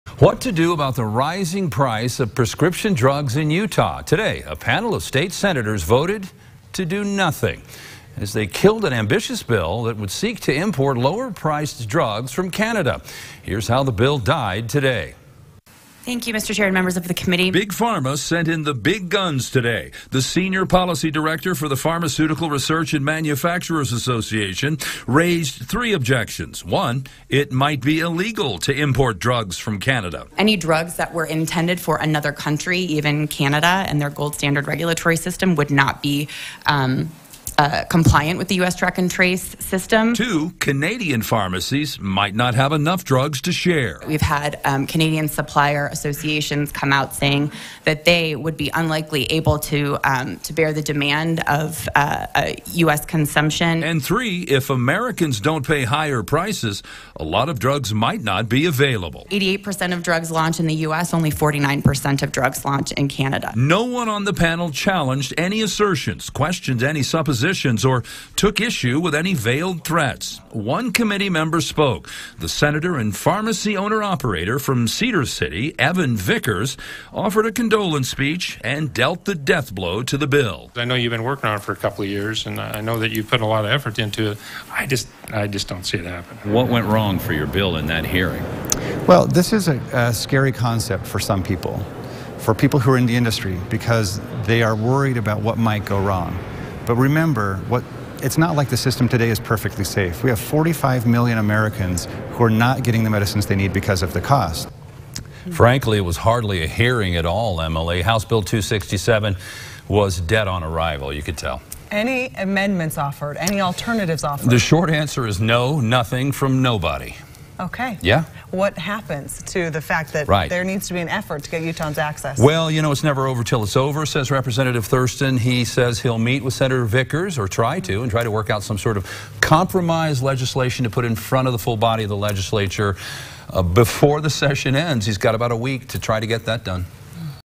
Canadian drug importation bill meets sudden death
Senate panel votes to do nothing about rising drug prices